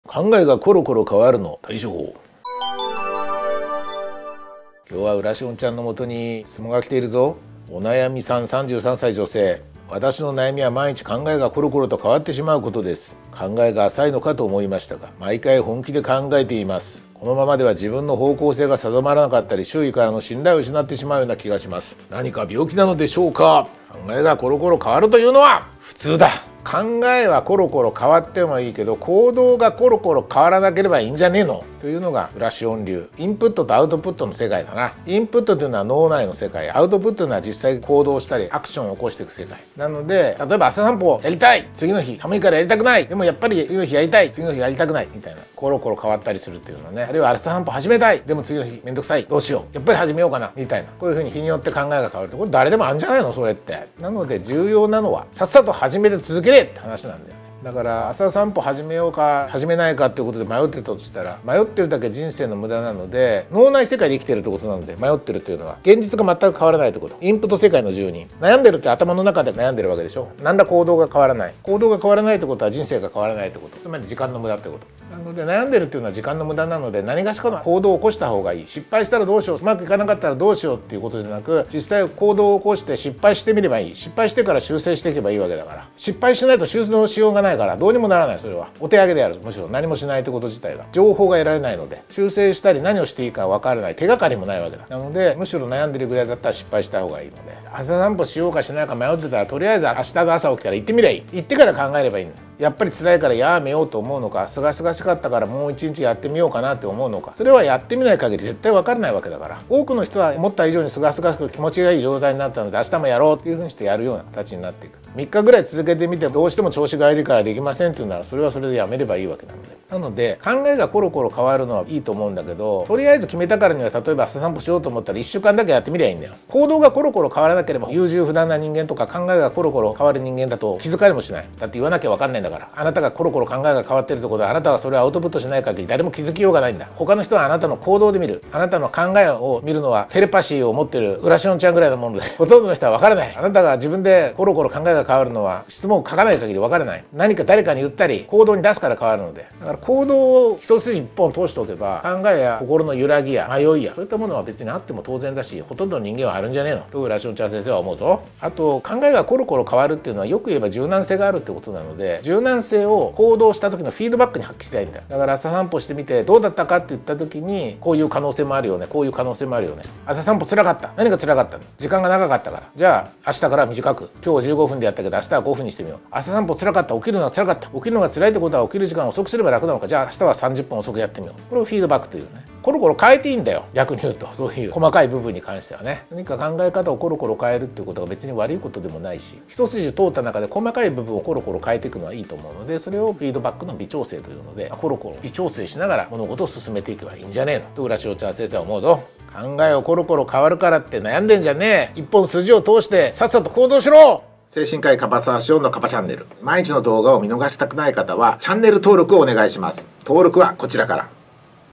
「考えがコロコロ変わる」の対処法【精神科医・樺沢紫苑】
【質問】「私の悩みは毎日考えがコロコロ変わってしまうことです。考えが浅いのかと思いましたが、毎回本気で考えています。
このままでは自分の方向性が定まらなかったり、周囲からの信用を失ってしまうような気がします。何か病気なのでしょうか？」
お悩みさん（33歳・女性）